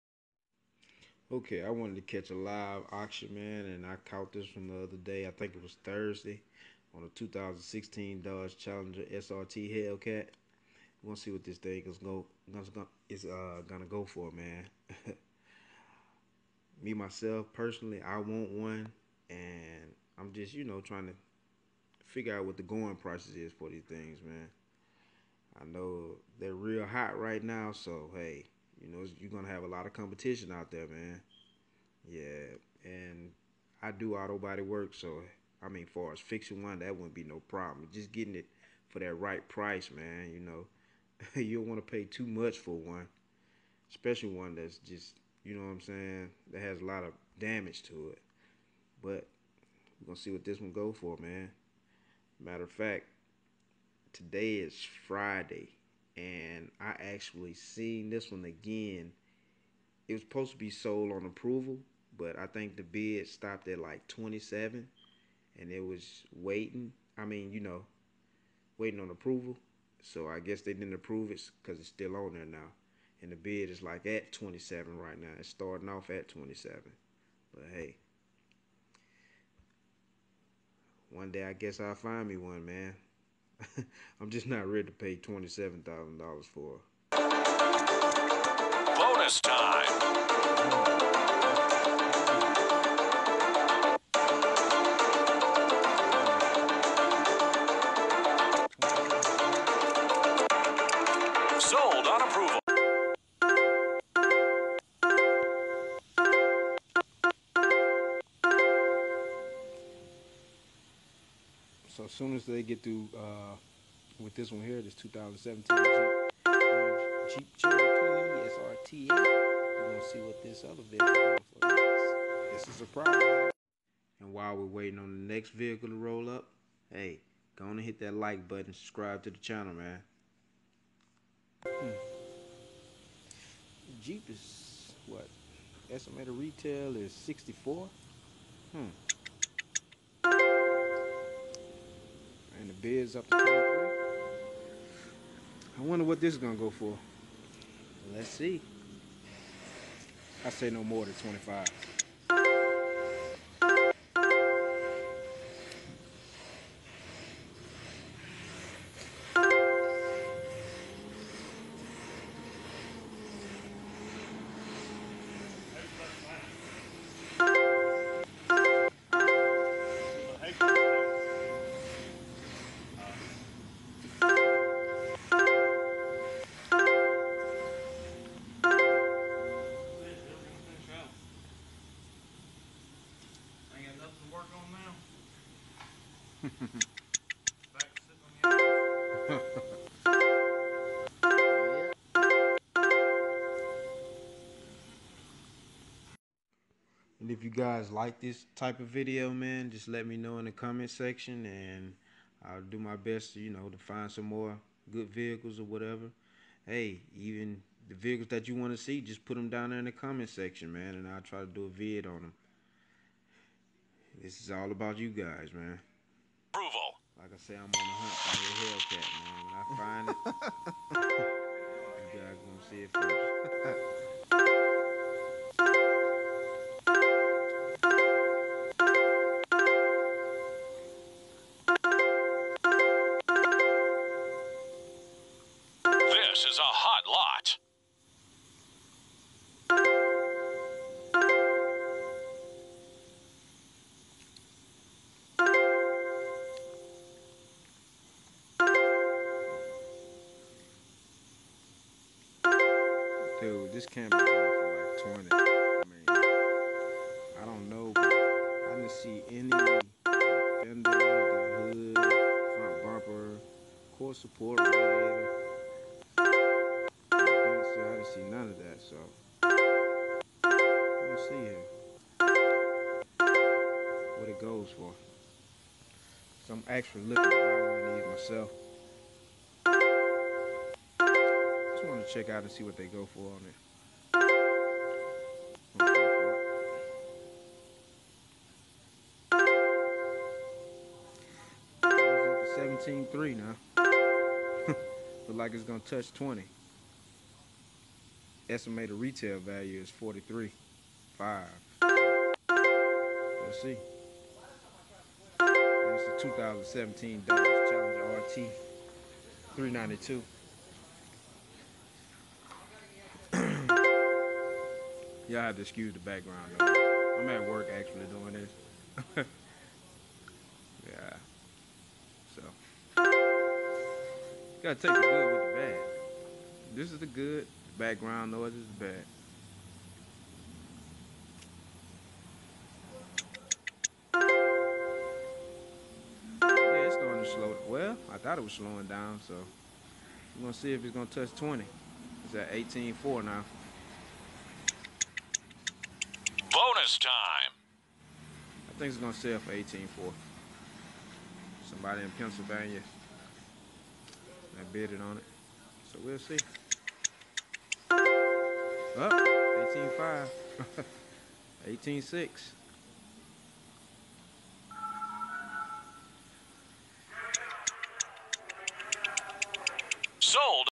Copart Live Auction 2016 Dodge Challenger Hellcat
Today we will be watching a live auction on Copart. Checking out what a wrecked Hellcat goes for at the auction.